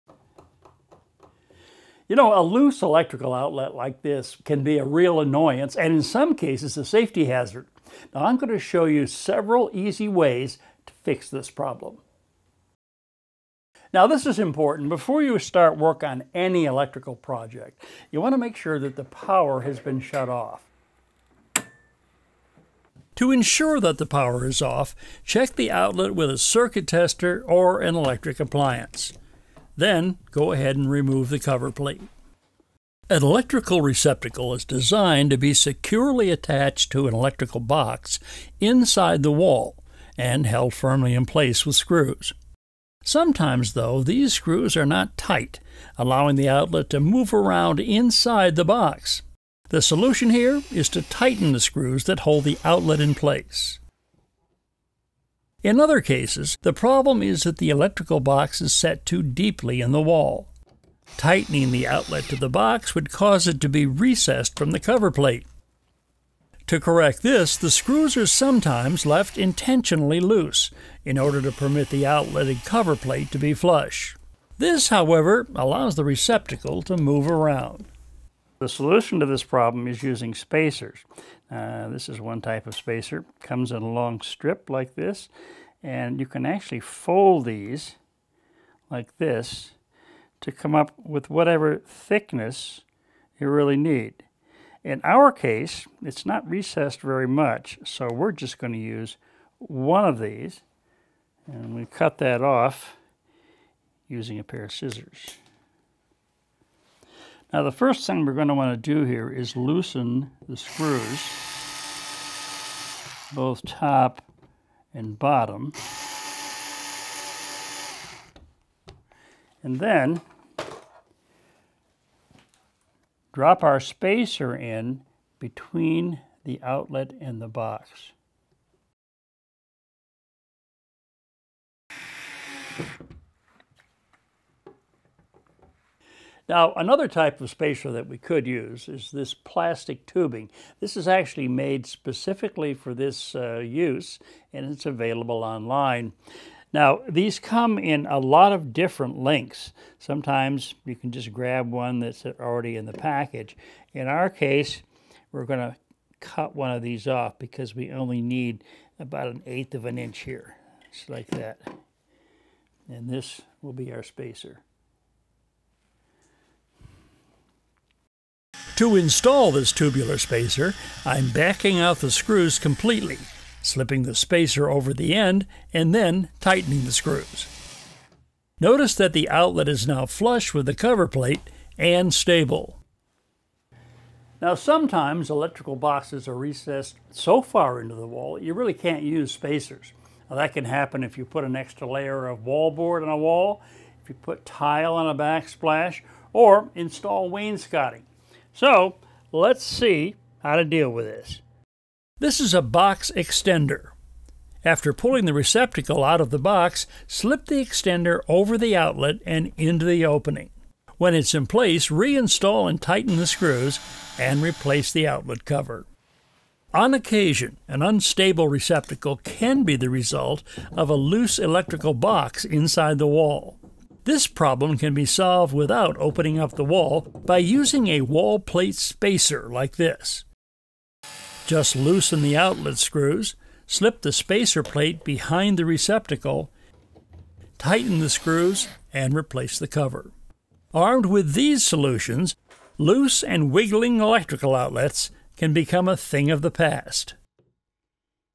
A Guide on How to Fix Loose Electrical Outlets
There are a few reasons your electrical outlets may become loose. These solutions will fix wobbly and annoying sockets for good.